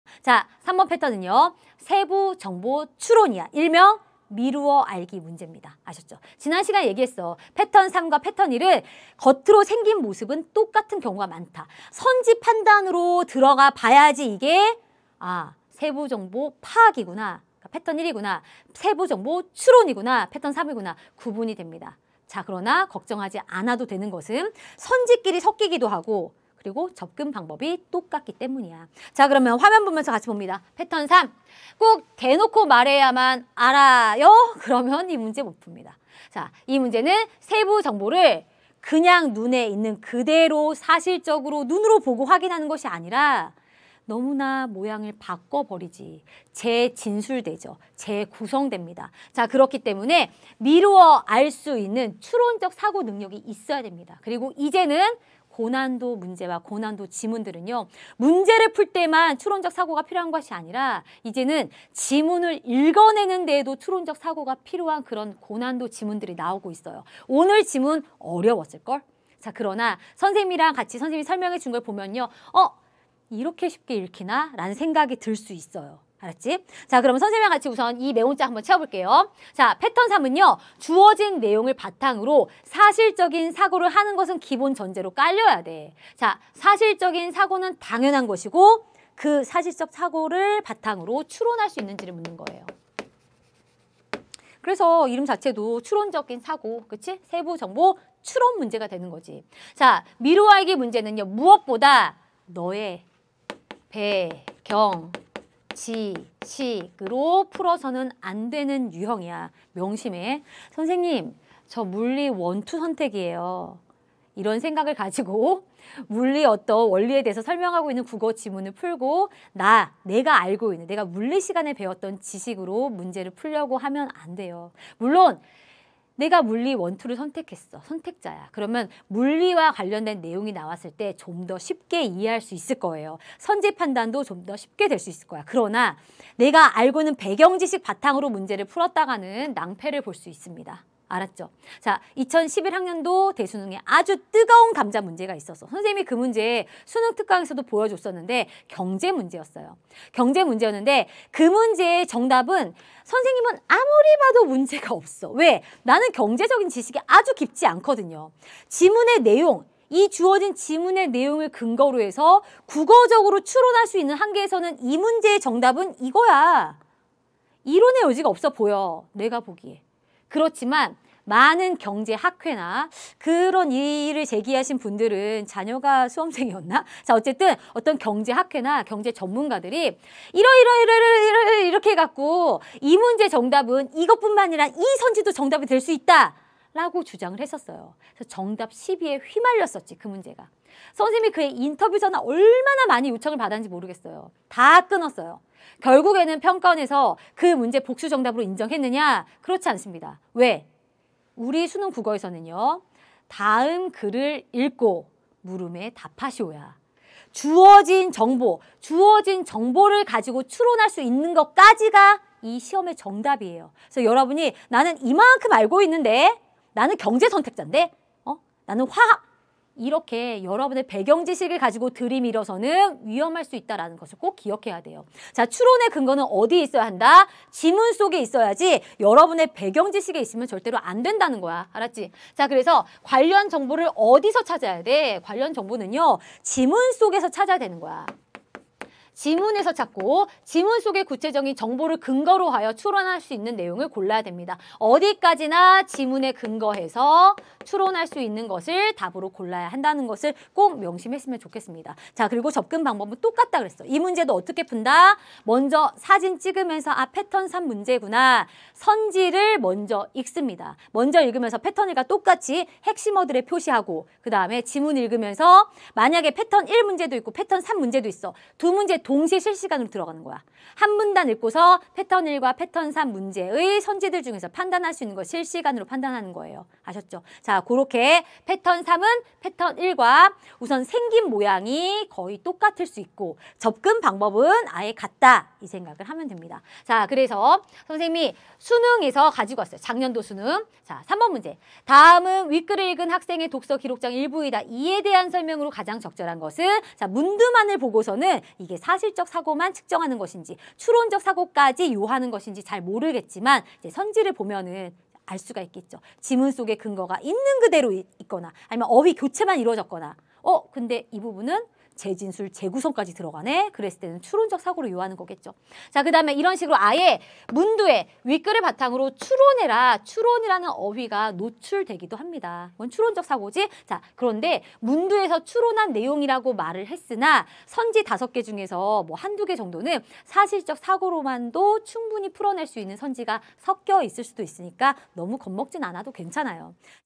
배경 지식으로 풀지마세요! 추론의 근거는 모두 지문속에 있어야 합니다 I 세부 정보 추론 문제 푸는 방법 I 만점 국어 I 윤혜정의 나비효과
올바른 국어 공부법만 안다면 국어 성적은 계단처럼 오른다!
전.설.의. 커리큘럼📚 문제 접근법을 정확하게 알려주는 윤혜정의 나비효과 패턴 3🦋